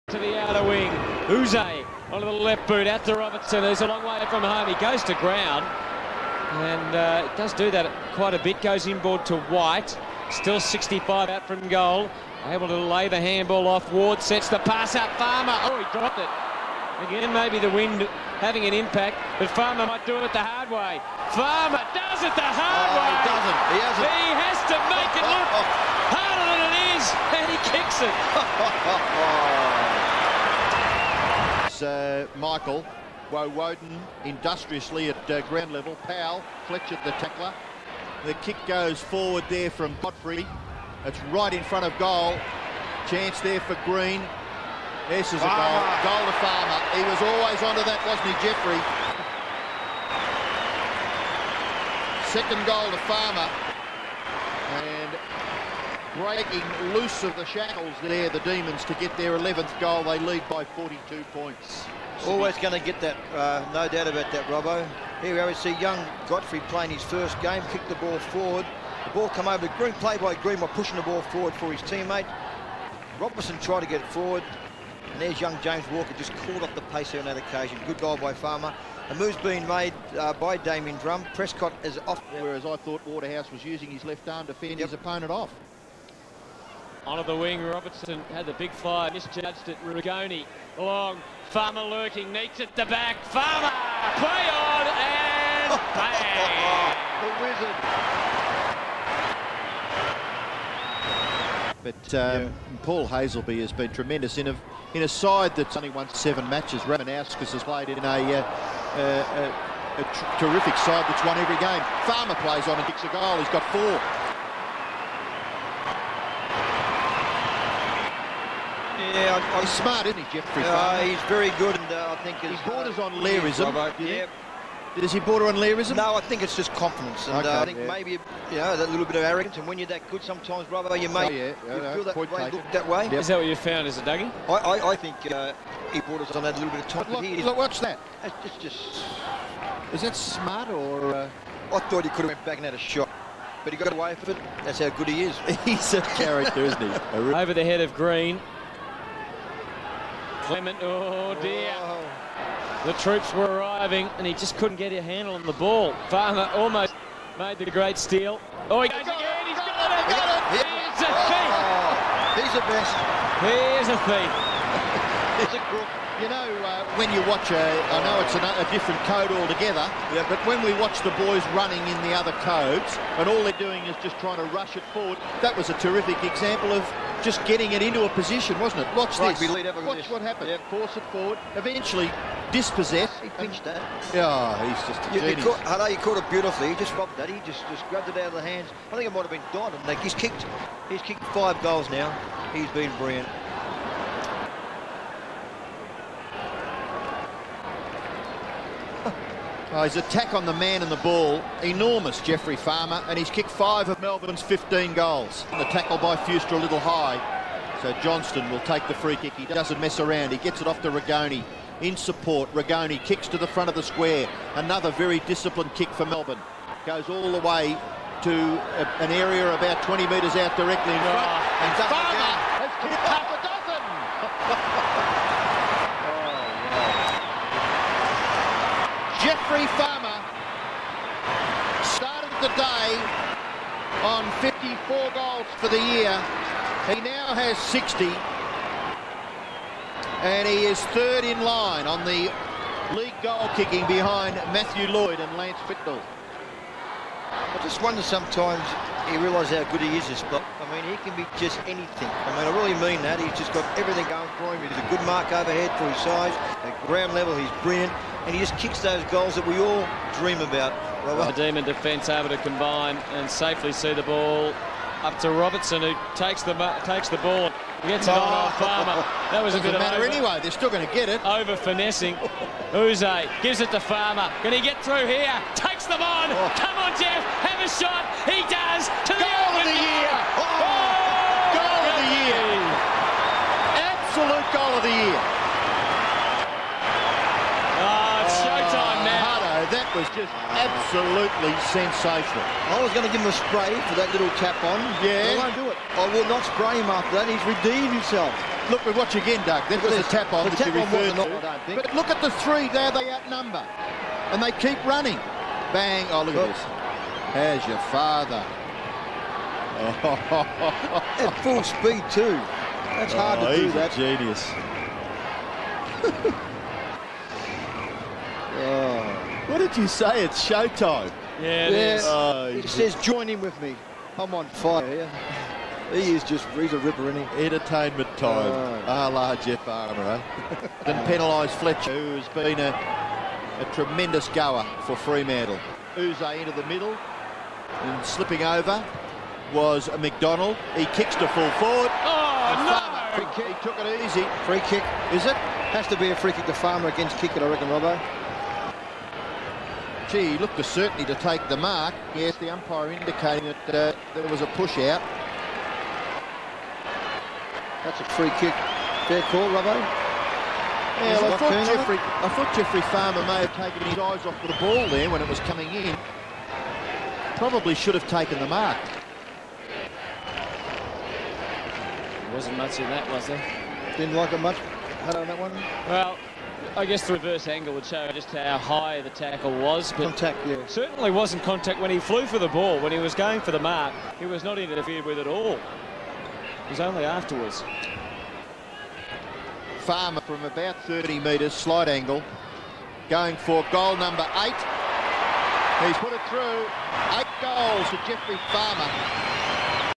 Jeff Farmer - Melbourne - 8 goals v Fremantle - 2000 Round 20 - Docklands - AFL
Jeff Farmer - Melbourne Demons - 8 goals v Fremantle - 2000 Round 20 - Docklands - AFL